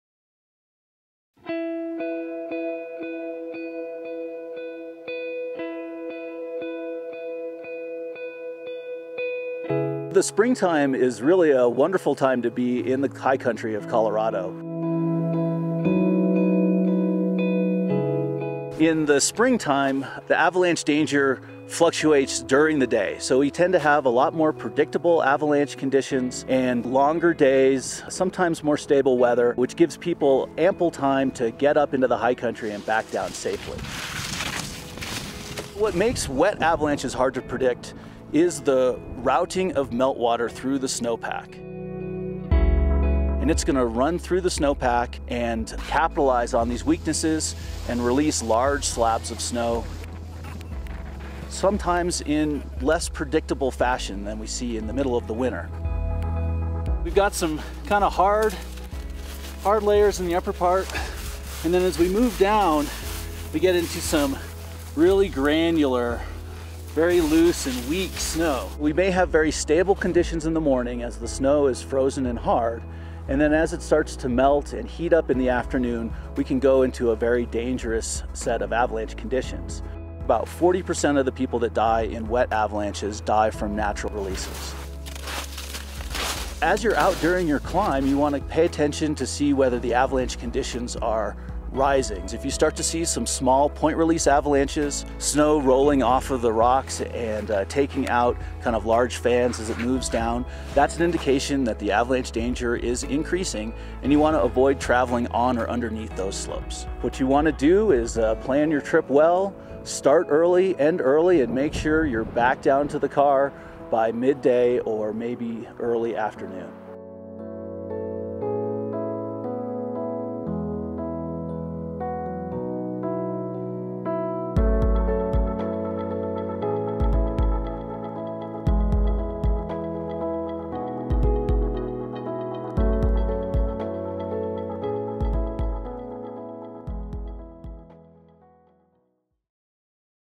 Spring Avalanche Hazards on Colorado's 14ers
Ethan Greene, director of the Colorado Avalanche Information Center, discusses the unique avalanche hazards posed in spring time on Colorado's 14,000-foot peaks. This is the time of the year when many 14er climbers are out doing snow routes that may become unsafe as the day progresses. Learn what causes spring avalanches and how you can look for signs of instability.